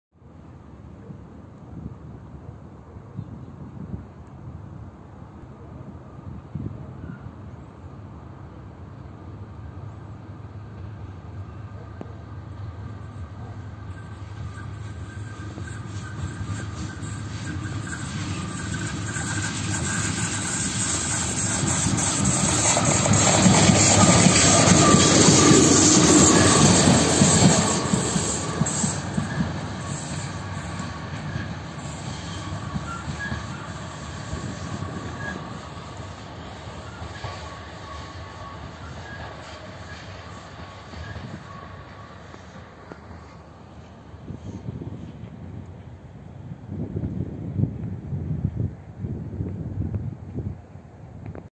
электровоз ВЛ11М6 идет резервом во тьме
Электровоз ВЛ11М6, перегон Встречный  -  Днепр-Лоцманский. 26.II.2021